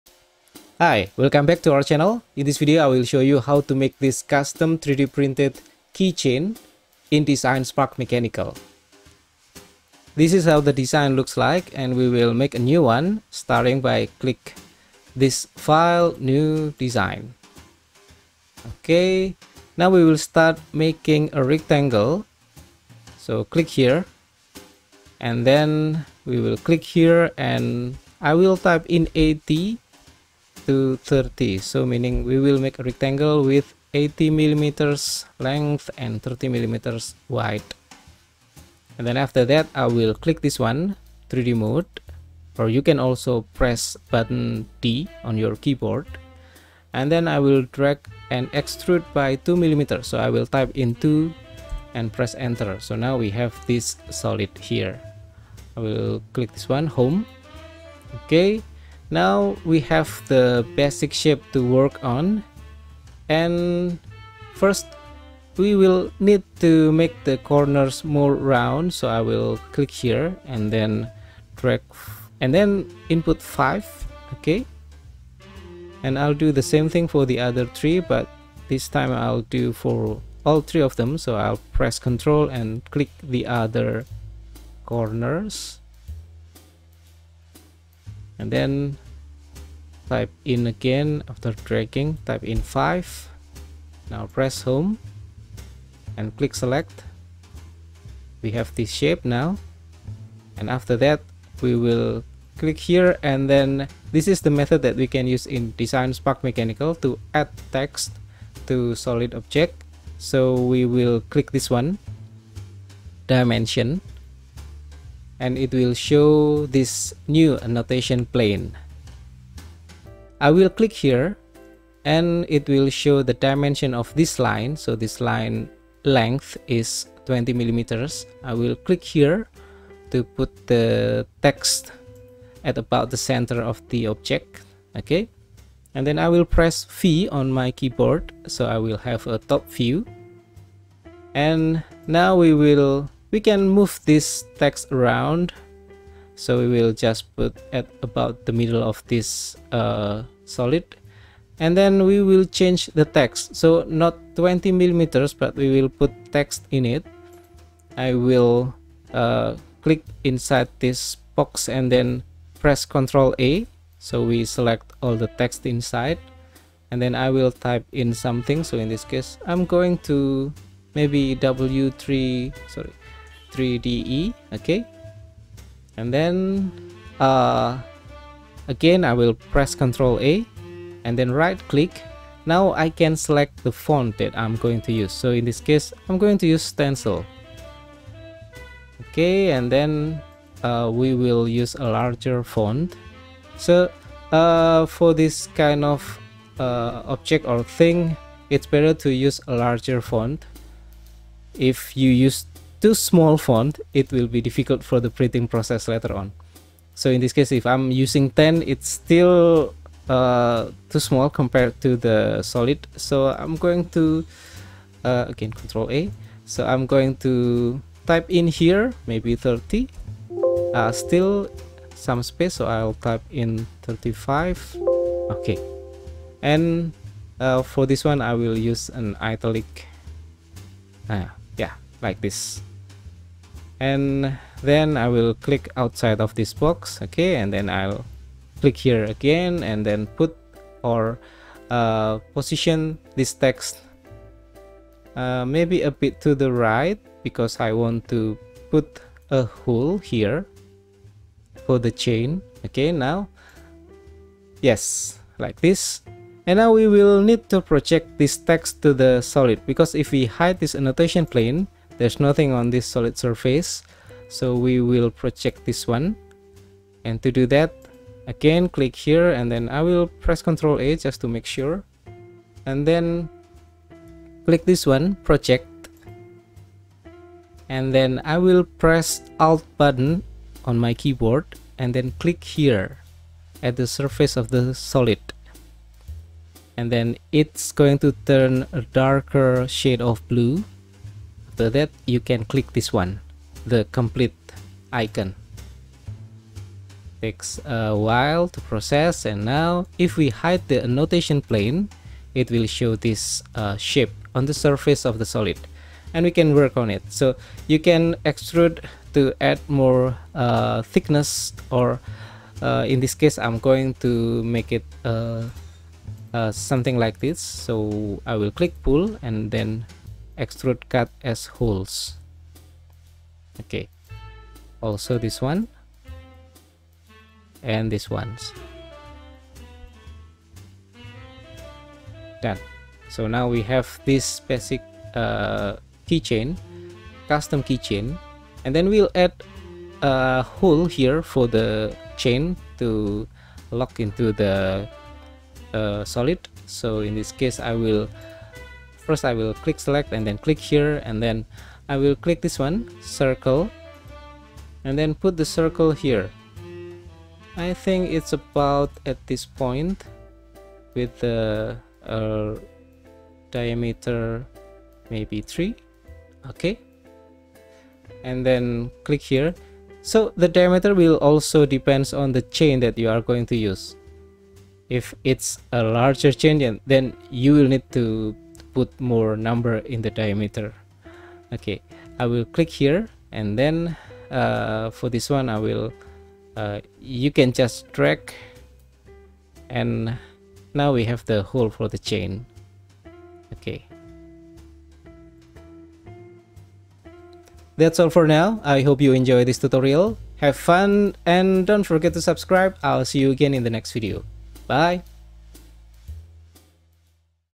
Designing Custom Text 3D Printable Keychain in Designspark Mechanical - DSM Narrated Tutorial
Designing Custom Text 3D Printable Keychain in Designspark Mechanical - DSM Modeling Narrated Tutorial

To export the design to STL or OBJ file, you can use the "Export Options" menu on the top right (in Design menu tab), and select STL as the format.

Yes, there are many mistakes in my grammar and limited (technical and non technical) vocabularies, please don't hesitate to correct me.